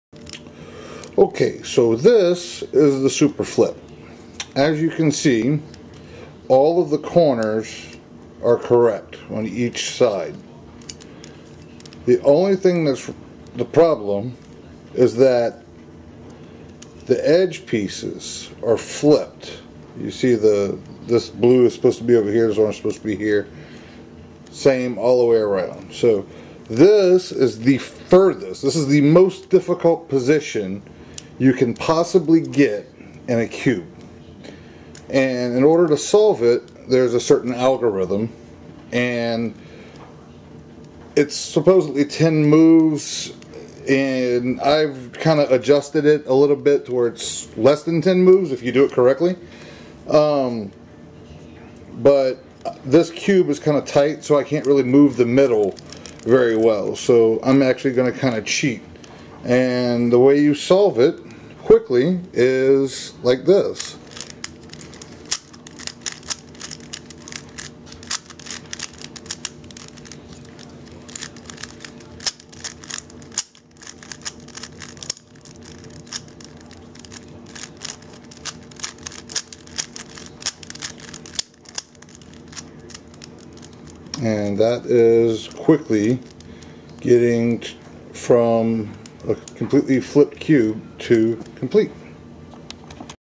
The Superflip solved
My friend Maria wanted to see the Superflip solved, so I threw this together for her. I know I said "10" moves in the video, but it's actually "20" moves. If I was able to move the middle slice easily on this cube, with the algorithm I used, it would have been 24 turns.

One optimal solution in HTM for the superflip is U R2 F B R B2 R U2 L B2 R U' D' R2 F R' L B2 U2 F2. The superflip also requires a minimum of 24 quarter turns to solve, and a minimum of 16 slice turns. One example of an optimal solution which requires 16 slice turns is S U B2 D2 M D' M2 S U R2 D M2 U B2 U S2. Another solution which requires 24 turns STM is MU'MU'MU'MU'yz'MU'MU'MU'MU'yz'MU'MU'MU'MU'. This is particularly easy to remember, as it is simply (MU')x4 followed by a yz'. This is repeated three times. This is basically the algorithm I used in the video, but instead of moving just the middle slice, I moved both the middle and the right, then just moved the right back to where it was (R').